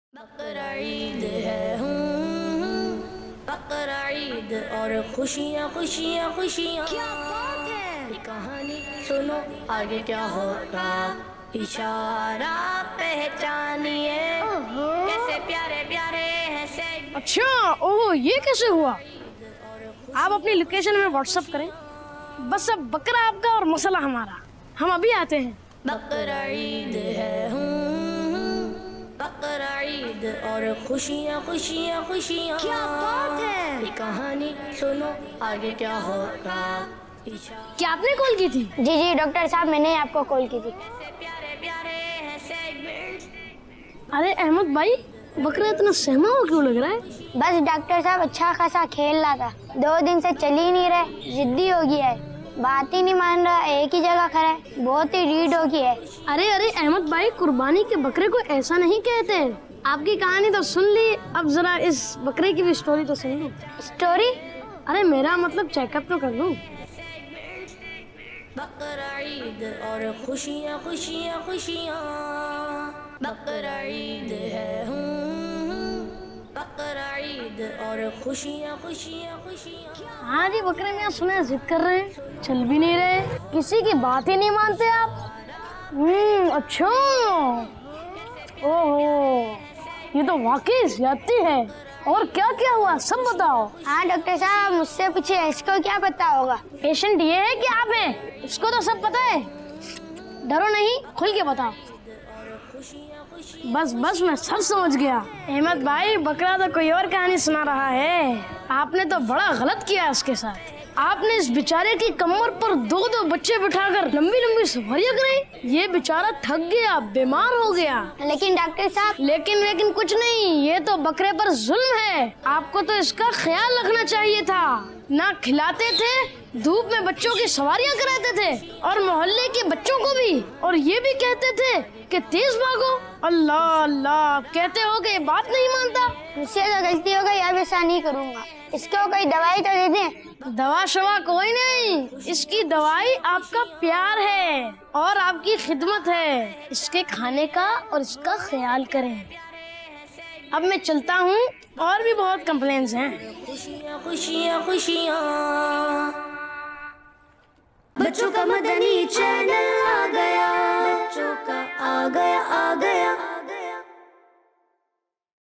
Dr Sahab Mera Bakra Chal Nahi Raha ☹️ | Mobile Doctor Bakra Clinic EP 01 | Bakra Eid 2021
Bakra Eid Special Video 2021: Dr Sahab Mera Bakra Chal Nahi Raha | Mobile Doctor Bakra Clinic EP 01 Bakra Clinic | Qurbani 2021 | Bakra Video | Bakra Eid 2021 in Pakistan - Kids Madani Channel

On the occasion of Eid ul Adha 2021, Kids Madani Channel has released Public Service Message for Kids "Mobile Doctor Bakra Clinic" , Like and Share this beautiful Kids Bakra Eid 2021 Story.